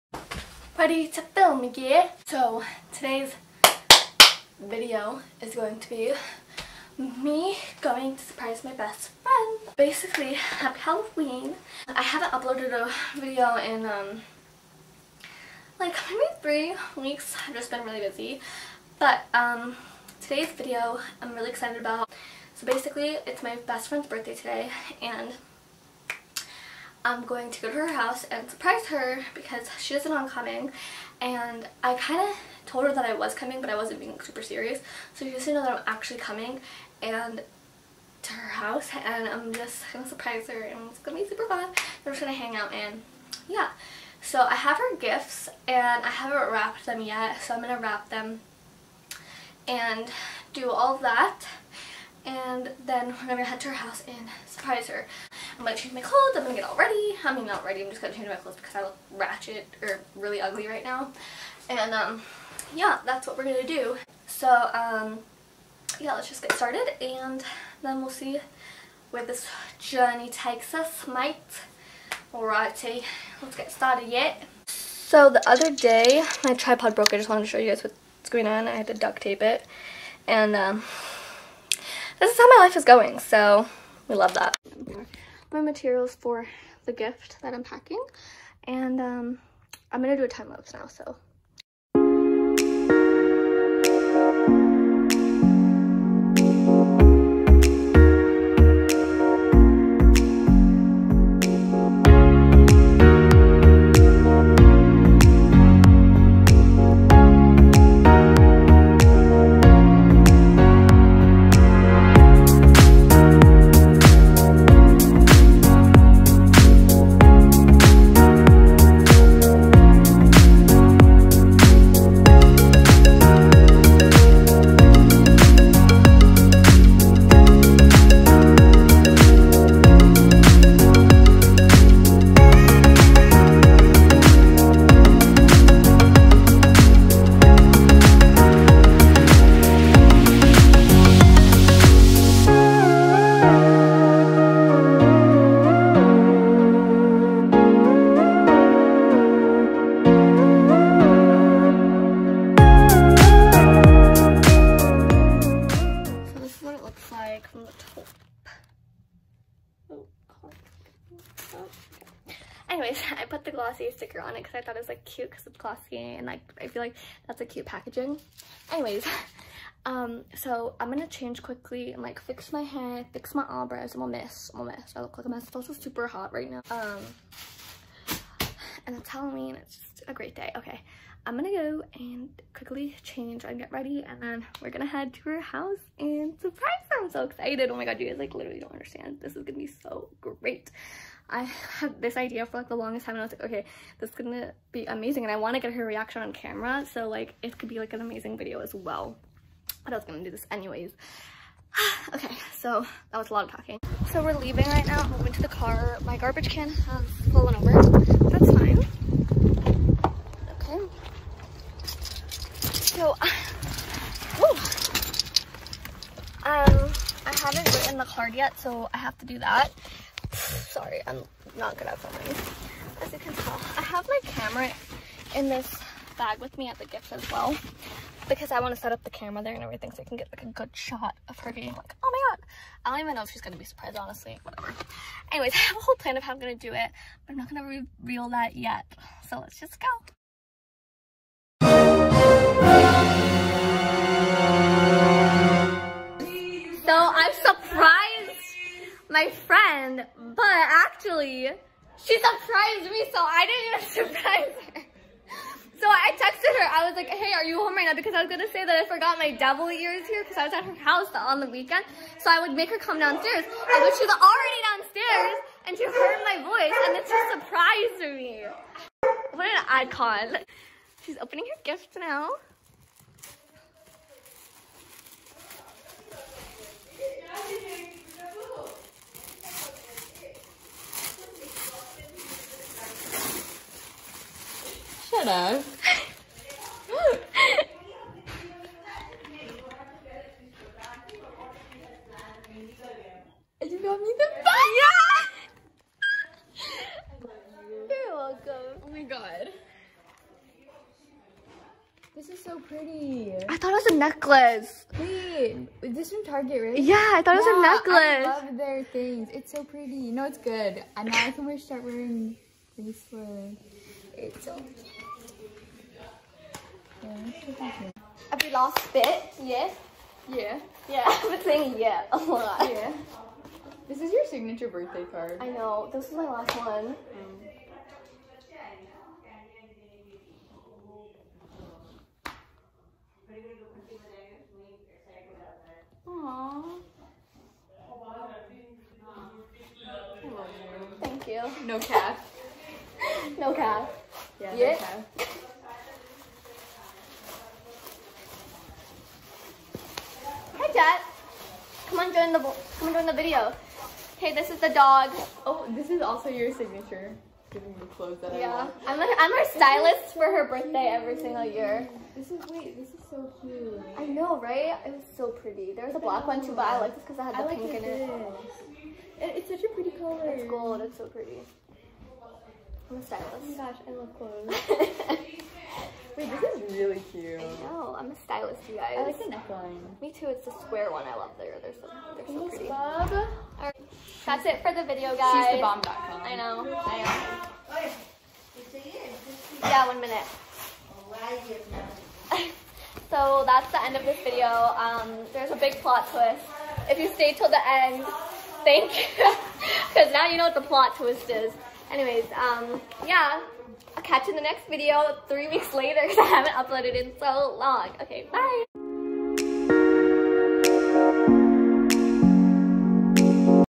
Surprising My Best Friend on Her Birthday!! *biggest plot twist EVER*
I know I haven't uploaded in probably a month, so thank you for being patient. I hope you enjoyed this video even with the HUGE plot twist! I loved "surprising" my best friend and spending time with her on her birthday and I hope you had fun watching this video!

instgram-- kenisha_seth
vsco-- kenishaseth